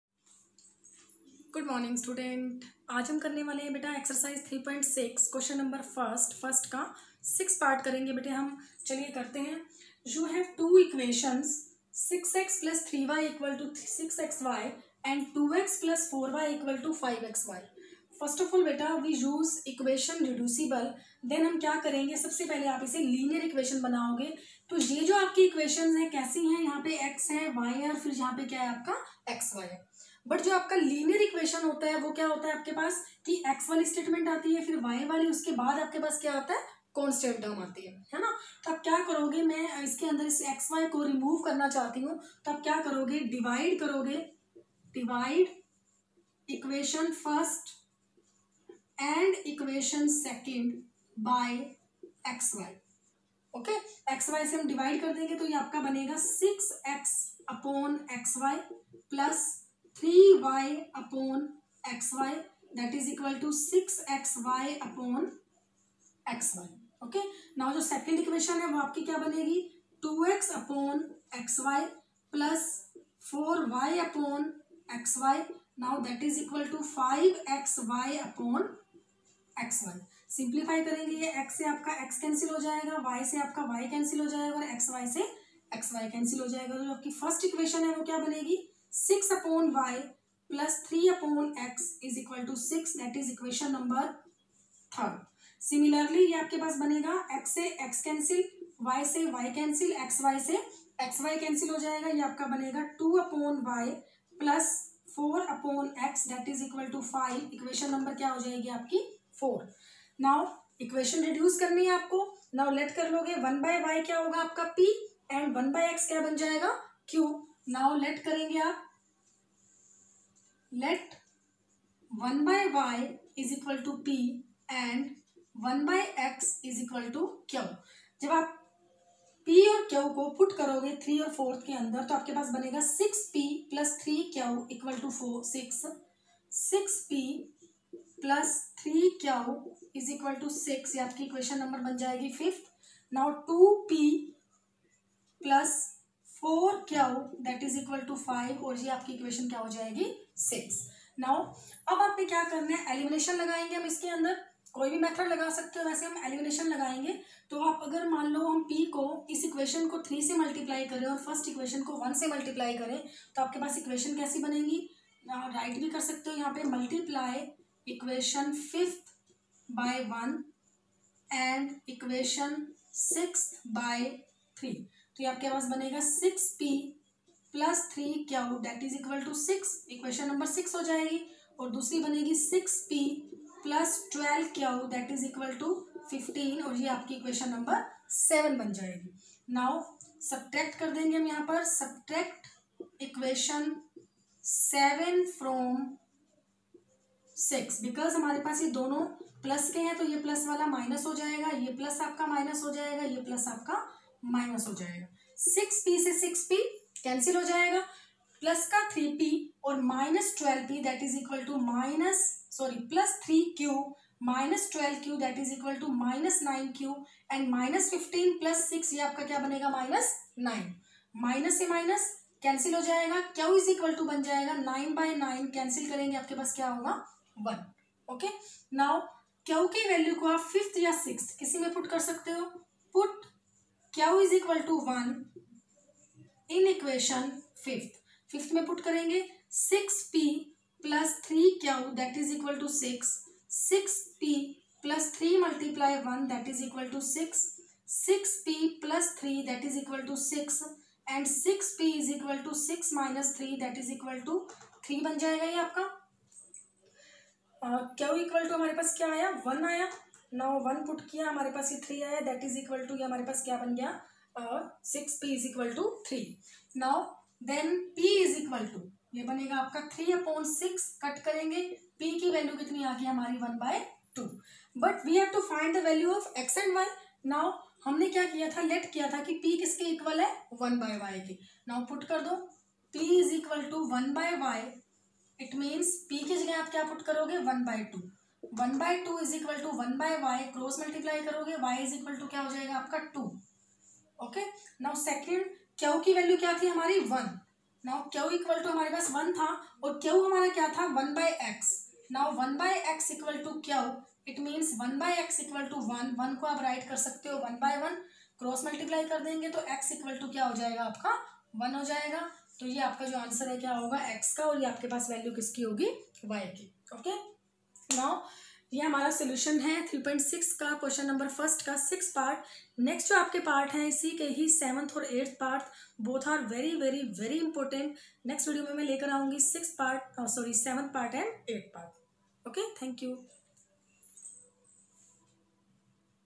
Maths 10th || Ch 3|| Pair of linear equations in two variables || Exercise 3.6|| Question 1(v¡)
Greetings for students
I am Pinki Kukreja welcome to all in my channel

👉 In this video we will learn about equations reducible and solutions of equations

I will upload all the videos of all chapters of 9th  and 10th ncert

👉 If you have any quary then ask me in comment box